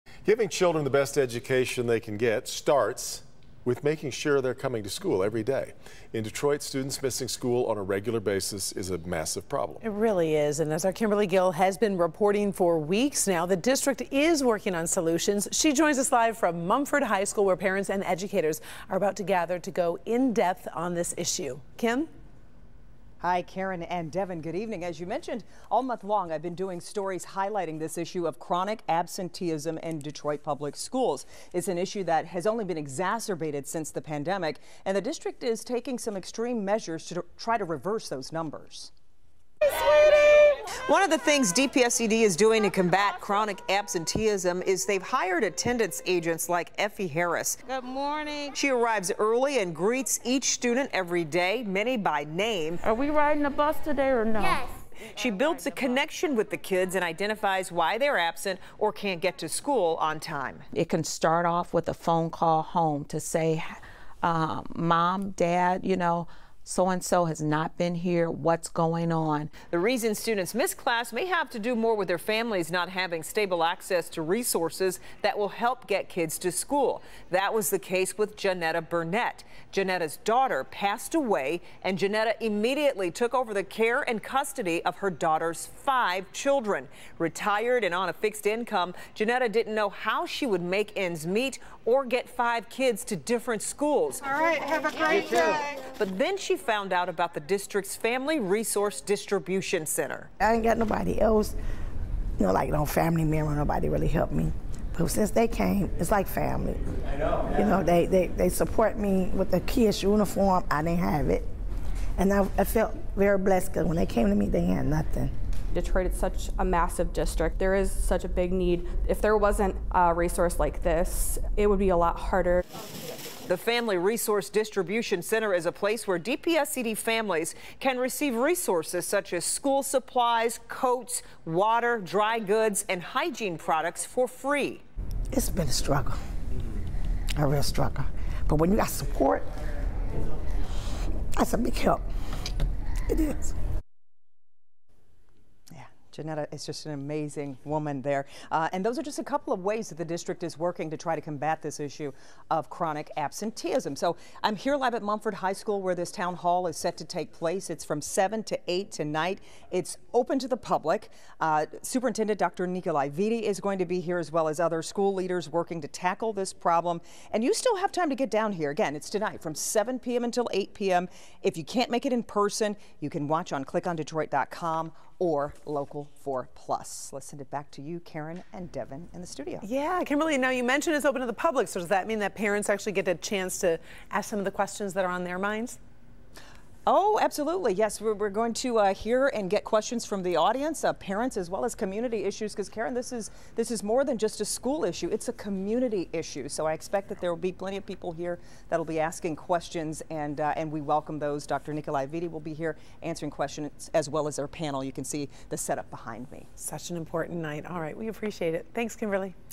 Here's how DPSCD is battling against chronic absenteeism
Giving children the best education they can get starts with ensuring they come to school daily. But in Detroit, students missing school regularly is a real problem.   I've been doing stories highlighting chronic absenteeism in Detroit Public Schools this month. It's a problem that's only been exacerbated since the pandemic, and the district is taking aggressive steps to reverse it.